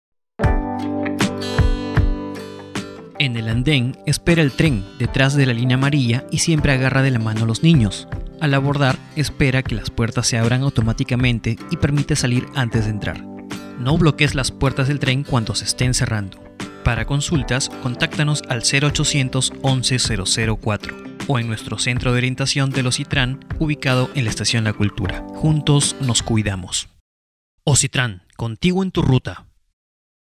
En el andén
Es muy importante estar atentos a las siguientes recomendaciones cuando esperamos el tren en el andén de la Línea 1 del Metro de Lima.